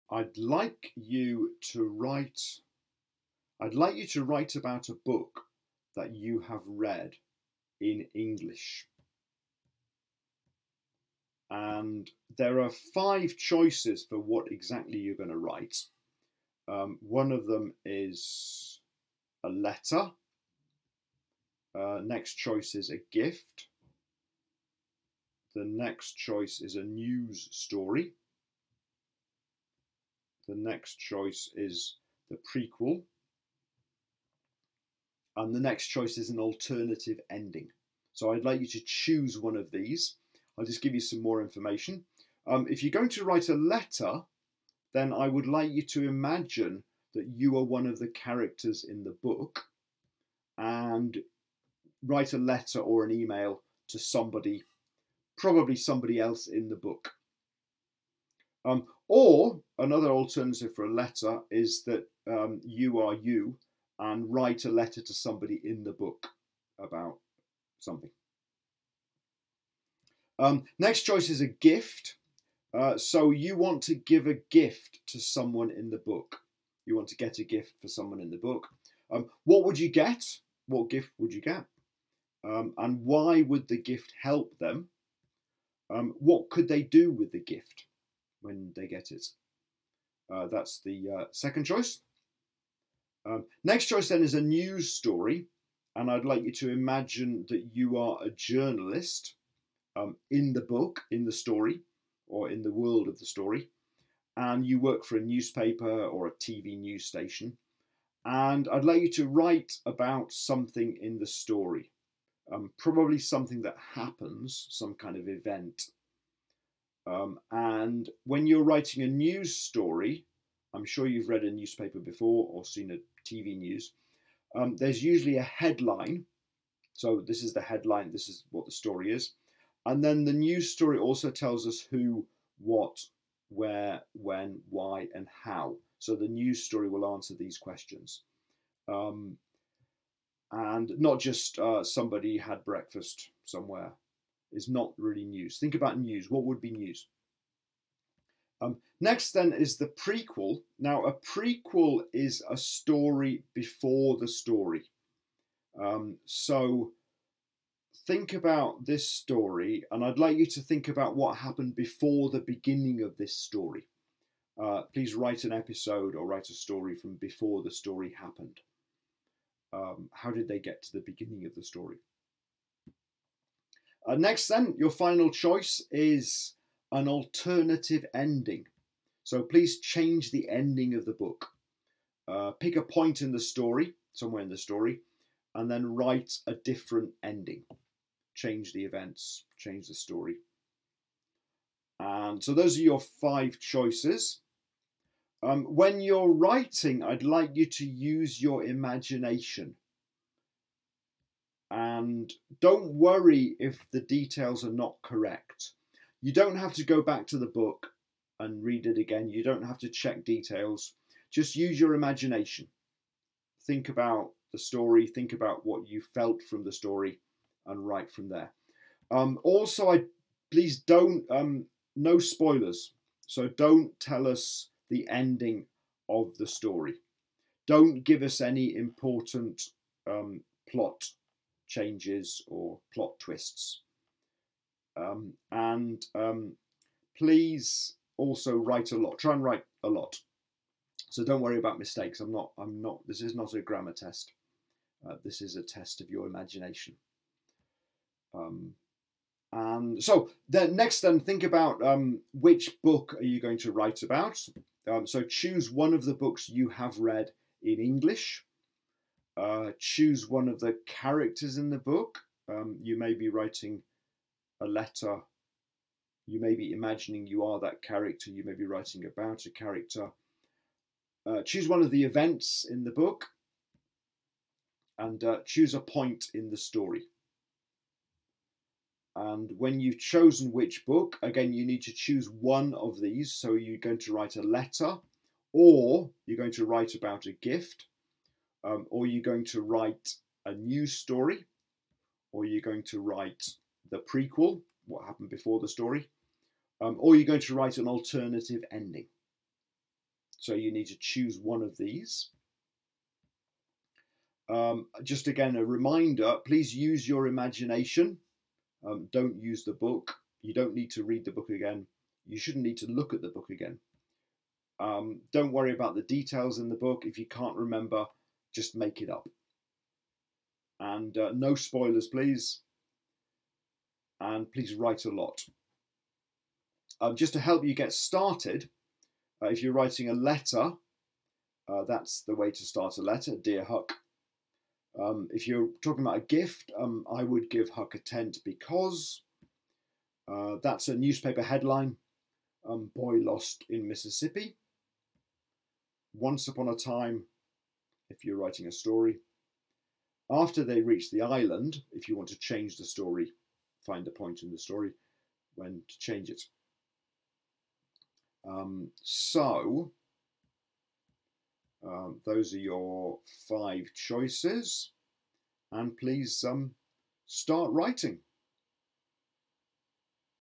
Writing about your Books: Part One
Write about a book you have read in English.
You have five choices:
- A letter
- About a Gift
- A News Story
- The Prequel
- An Alternative Ending